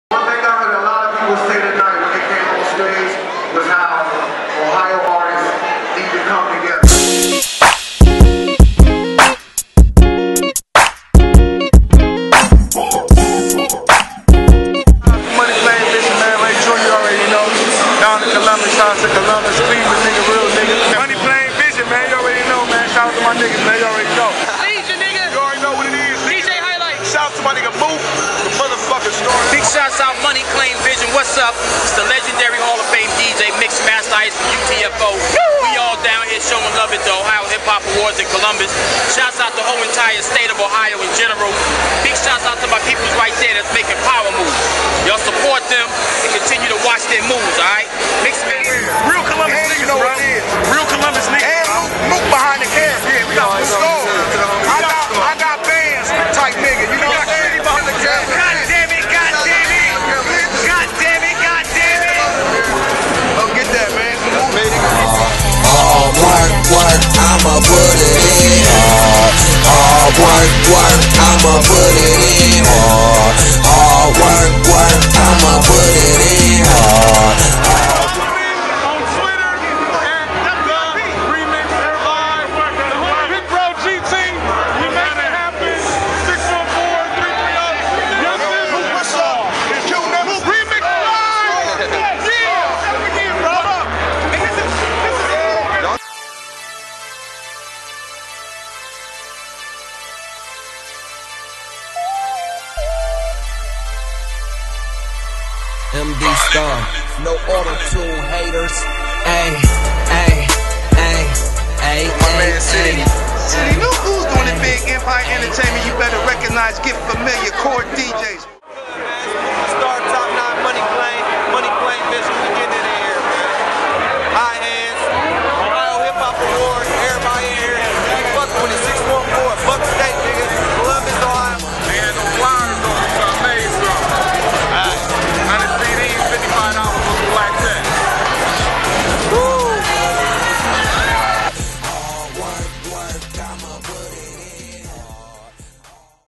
OHIO HIP HOP Awards PART 1 (Money Plane ViZiON)
OHIO HIP - HOP AWARDS IN COLUMBUS, OHIO.. FILMED & EDITED BY: "MONEY PLANE VIZION"

NEED A "MUSIC VIDEO" HIT US UP.. MPV